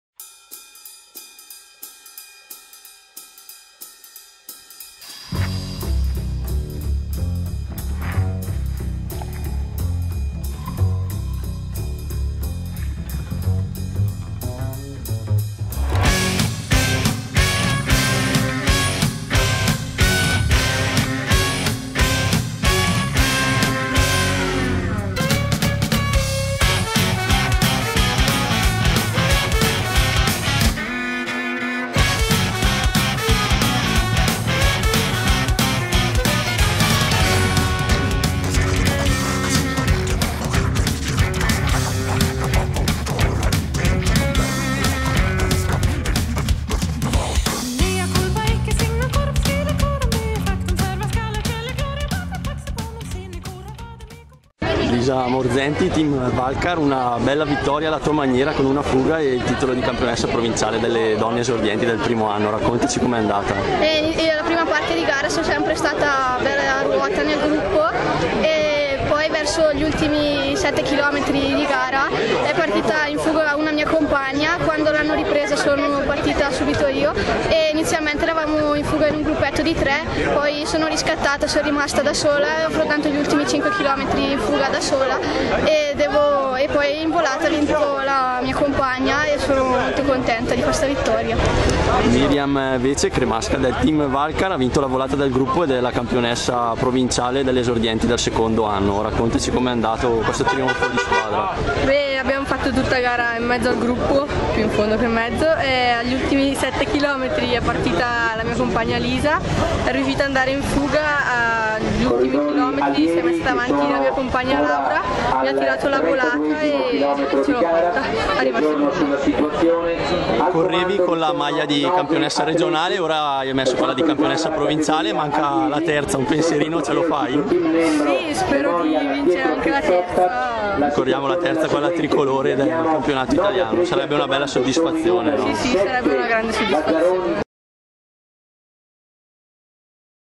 Campionato Provinciale Donne Esordienti
Campionato Provinciale Donne Esordienti - vittoria di Lisa Morzenti (Team Valcar), 2° la compagna Miriam Vece e sono rispettivamente campionesse bergamasche Donne Esordienti 1° e 2° anno.